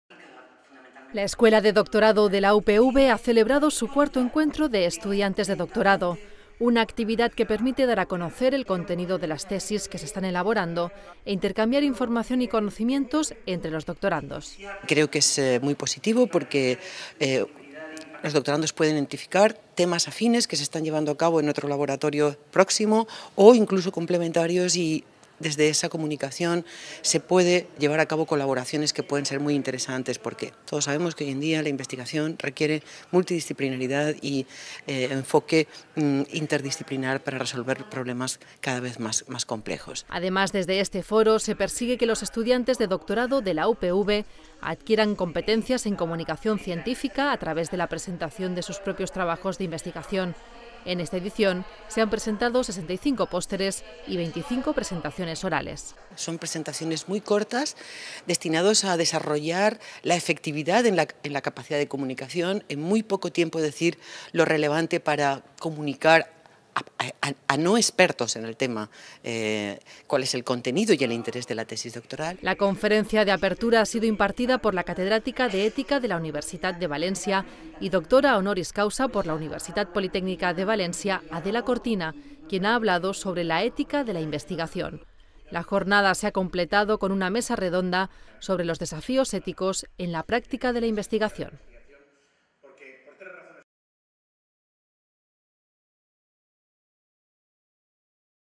IV Encuentro de Estudiantes de Doctorado - Noticia @UPVTV 01-06-2017
La Escuela de Doctorado de la UPV ha celebrado su cuarto Encuentro  de Estudiantes de doctorado. Una actividad que permite dar a conocer el contenido de las tesis que se están elaborando e intercambiar información y conocimientos entre los doctorandos.

Además, desde este foro, se persigue que los estudiantes de doctorado de la UPV adquieran competencias en comunicación científica a través de la presentación de sus propios trabajos de investigación. En esta edición se han presentado 65 pósteres y 25 presentaciones orales.

La conferencia de apertura ha sido impartida por la catedrática de ética de la Universitat de València y doctora Honoris Causa por la UPV, Adela Cortina, quien ha hablado sobre la "Ética de la investigación".

La jornada se ha completado con una mesa redonda sobre los "Desafíos éticos en la práctica de la investigación".